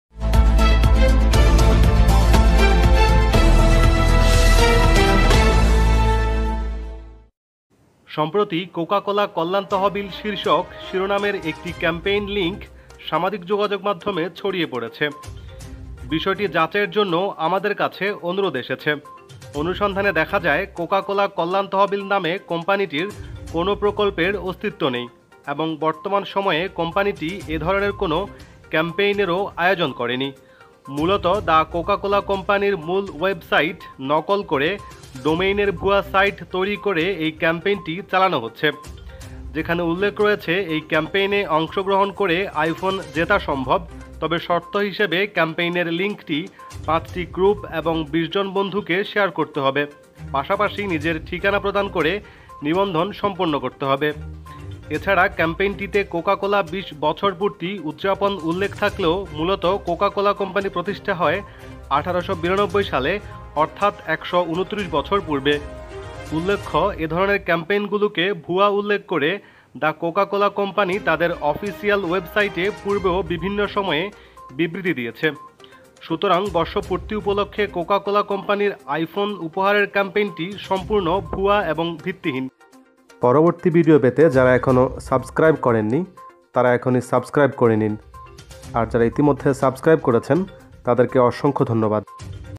কোকাকোলা কল্যাণ তহবিল! পাবেন আইফোন?। Coca-Cola campaign। bd tv news
সম্প্রতি “কোকা কোলা কল্যাণ তহবিল” শীর্ষক শিরোনামের একটি ক্যাম্পেইন লিংক সামাজিক যোগাযোগ মাধ্যমে ছড়িয়ে পড়েছে।
অনুসন্ধানে দেখা যায় ‘কোকা কোলা কল্যাণ তহবিল’ নামে কোম্পানিটির কোন প্রকল্পের অস্তিত্ব নেই এবং বর্তমান সময়ে কোম্পানিটি এধরণের কোন ক্যাম্পেইনেরও আয়োজন করেনি।
মূলত দ্যা কোকা কোলা কোম্পানির মূল ওয়েবসাইট
ডোমেইনের ভুয়া সাইট তৈরি করে এই ক্যাম্পেইনটি চালানো হচ্ছে।
যেখানে উল্লেখ রয়েছে এই ক্যাম্পেইনে অংশগ্রহণ করে আইফোন জেতা সম্ভব তবে শর্ত হিসেবে ক্যাম্পেইনের লিংকটি ৫ টি গ্রুপ এবং ২০ জন বন্ধুকে শেয়ার করতে হবে পাশাপাশি নিজের ঠিকানা প্রদান করে নিবন্ধন সম্পন্ন করতে হবে।
এছাড়া ক্যাম্পেইনটিতে “কোকা কোলা বিশ বছর পূর্তি উদযাপন” উল্লেখ থাকলেও মূলত কোকা কোলা কোম্পানি প্রতিষ্ঠা হয় ১৮৯২ সালে অর্থাৎ ১২৯ বছর পূর্বে।
উল্লেখ্য, এধরণের ক্যাম্পেইনগুলোকে ভুয়া উল্লেখ করে দ্যা কোকা কোলা কোম্পানি তাদের অফিসিয়াল ওয়েবসাইটে পূর্বেও বিভিন্ন সময়ে বিবৃতি দিয়েছে।